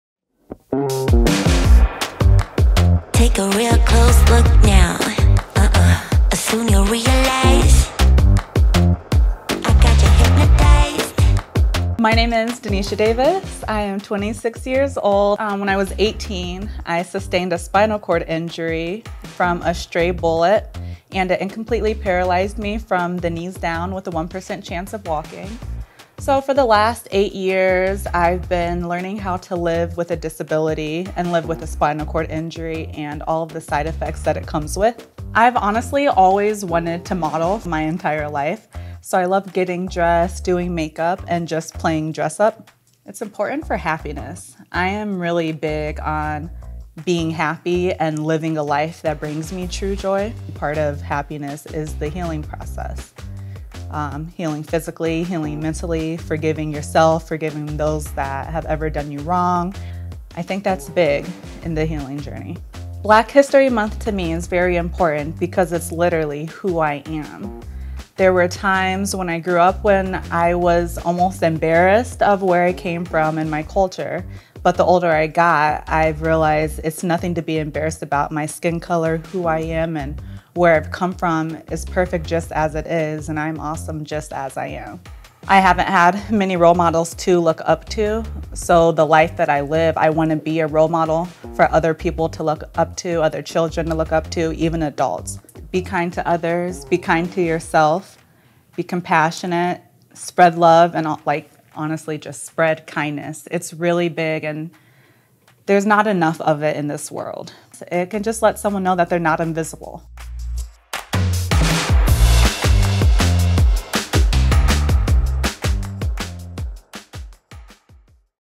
Black Model with a Disability | Black History Month with Denisha Davis
Denisha Davis, 26, wears many shades. She is a survivor of gun violence. She is a wheelchair user with a love for fashion and fitness. She is a model, an influencer and she is proudly black.

Davis says she aspires to be a role model for all and has used social media platforms such as Instagram to outline her journey. On her Instagram (@_milkchocolateonwheelz), you'll find everything from adaptive workouts to beauty tips.

For Black History Month, Davis sat down with us to celebrate her story and culture.

And be sure to stay up to date with Ability360, a non-profit center for independent living based in Arizona.